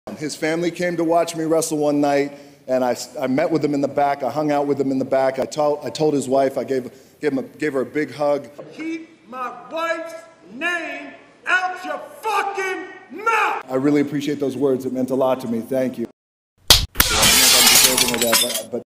The Rock slaps Will Smith
I made the Rock slap Will Smith in davinci resolve!